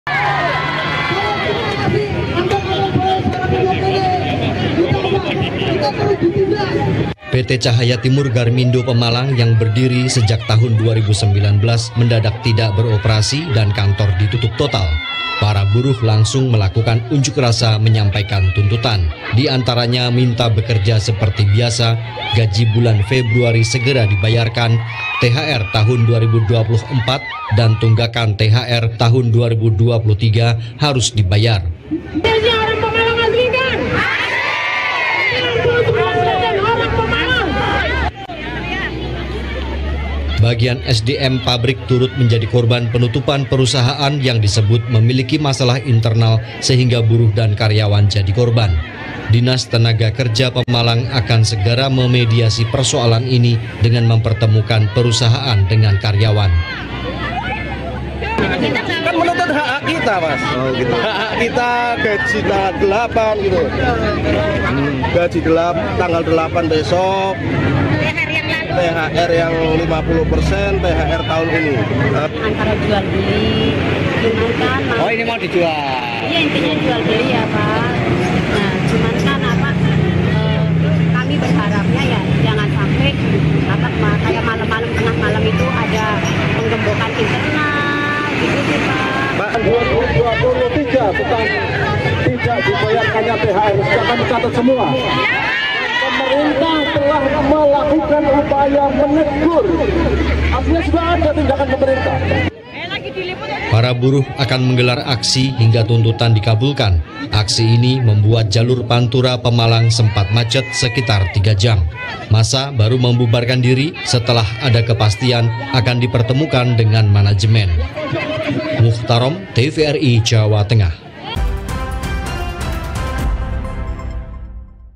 Pabrik Ditutup, Ratusan Pekerja Unjuk Rasa di Pemalang
JatengTerkini 8 Maret 2024

Ratusan buruh PT. Cahaya Timur Garmindo Pemalang berunjuk rasa di depan pintu masuk perusahaan. Karyawan yang datang kaget karena pintu pabrik tiba-tiba digembok.